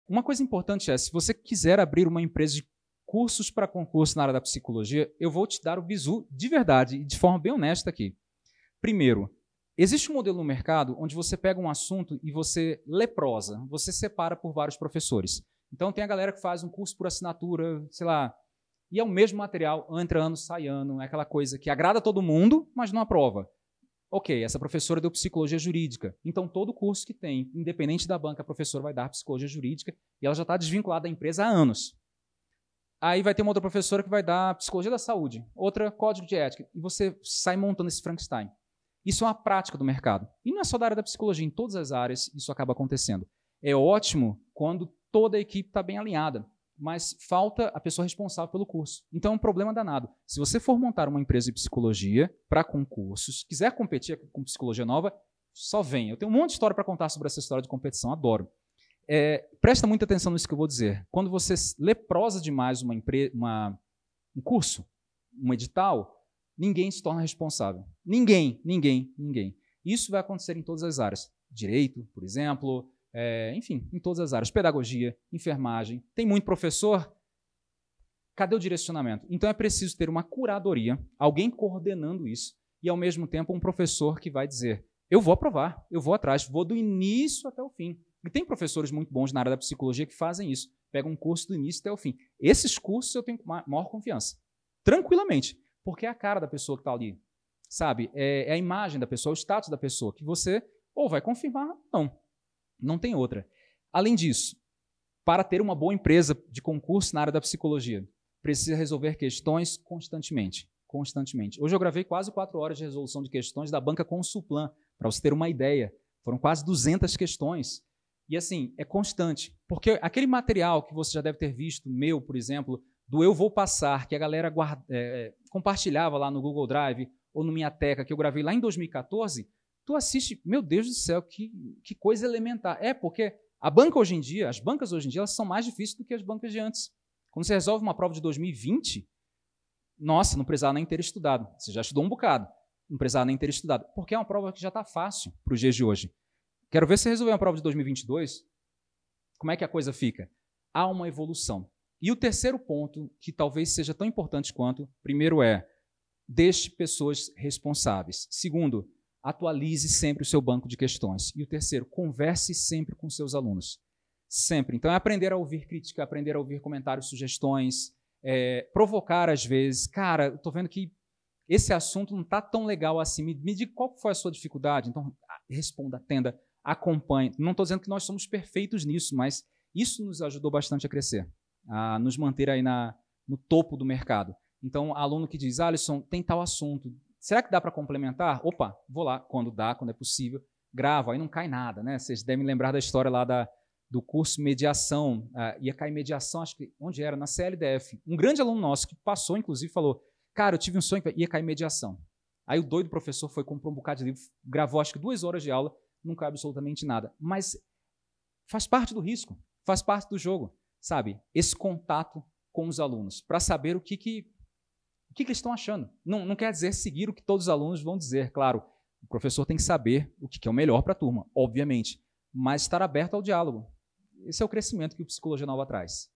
Como abrir uma empresa de cursos para concursos de psicologia
Histórias dos bastidores do Psicologia Nova